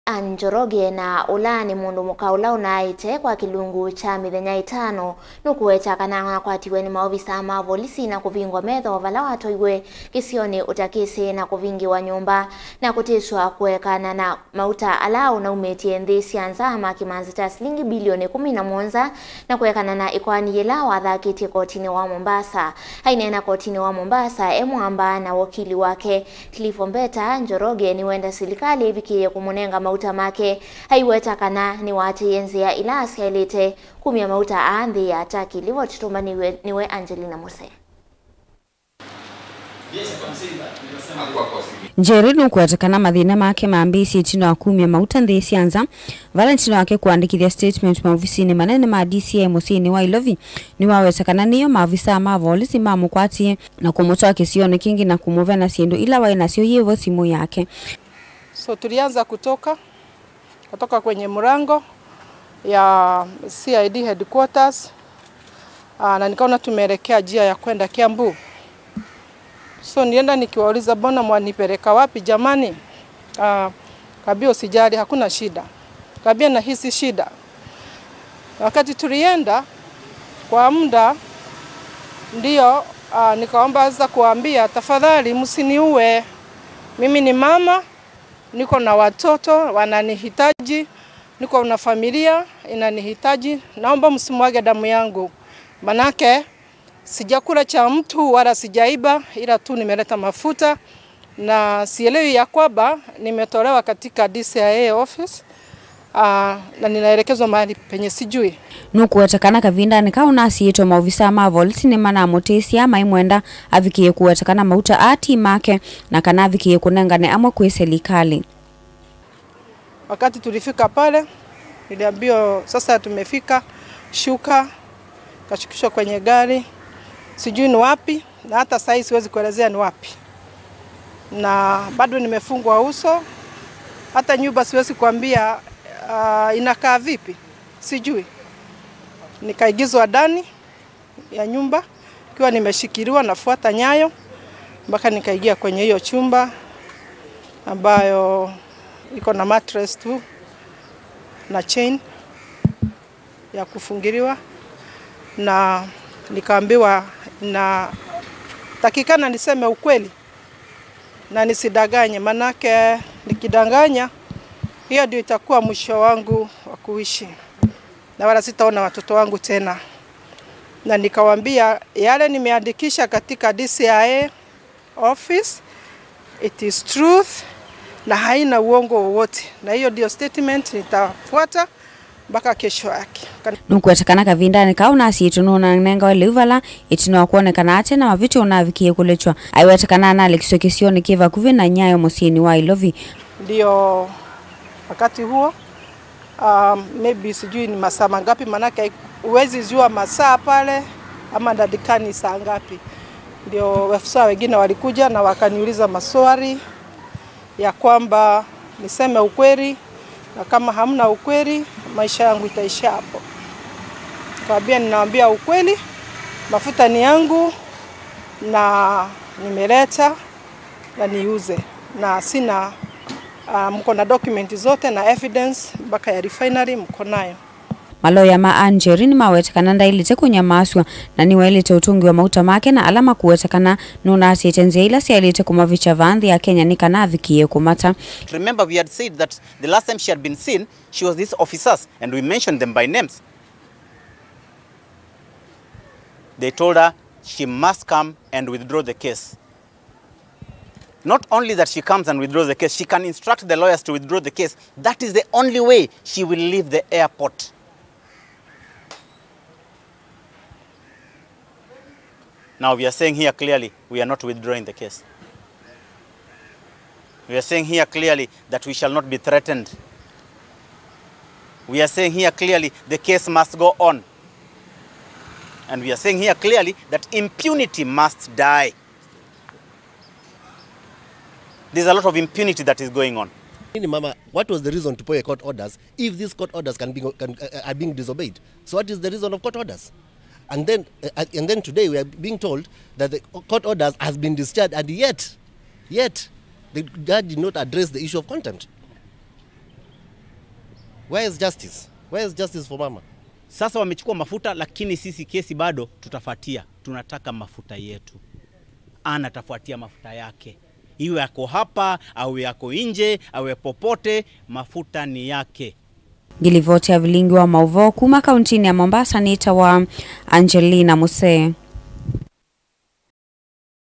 MUTANDITHYA ANNE NJOROGE KUKWATIKANA
Ann Njoroge na ula ni mundumuka ula unaite kwa kilungu kya mithenya itano nukuweta kana akwatiwe ni maovisaa ma volisi na kuvingwa metho vala watwaiwe kisioni utakisi na kuvingiwa  nyumba na kutiswa kuekana na mauta ala waumitye nthi sya nza ma kimanza kya silingi bilioni 17 na kwekana na ikoani yila wathakitye kotini wa mombasa. Ainenea kotini wa mombasa e muamba na wakili wake Cliff Ombeta, Anne niwenda silikali kumunenge mauta make aiweta kana niwatiite nzia ila syailite kumya mauta aa nthi ya Turkey.